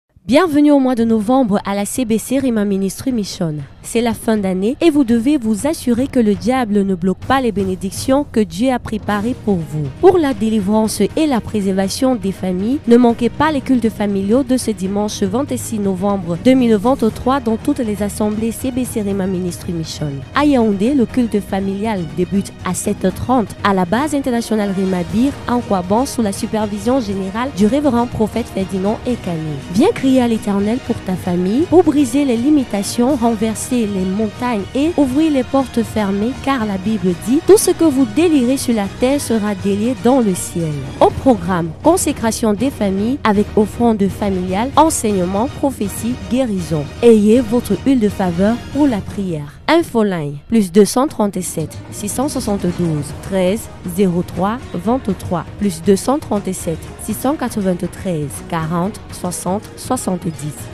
⛔️NE MANQUEZ PAS LE GRAND CULTE FAMILIAL CE DIMANCHE 26 NOVEMBRE 2023 DES 7H30 À LA BIR
⛔️NE MANQUEZ PAS LE GRAND CULTE FAMILIAL
📌CE DIMANCHE 26 NOVEMBRE 2023 ⏰ DES 7H30
👉🏾À LA BASE INTERNATIONALE RHEMA BIR NKOABANG
👉🏾DANS TOUTES LES ASSEMBLÉES RHEMA AUX HEURES HABITUELLES
🔥AVEC LE REV.PROPHÈTE FERDINAND EKANE

𝐏𝐎𝐔𝐑 𝐕𝐎𝐒 𝐃Î𝐌𝐄𝐒  𝐄𝐓 𝐎𝐅𝐅𝐑𝐀𝐍𝐃𝐄𝐒 :
▪️𝗠𝗢𝗕𝗜𝗟𝗘 𝗠𝗢𝗡𝗘𝗬

SOYEZ BÉNI.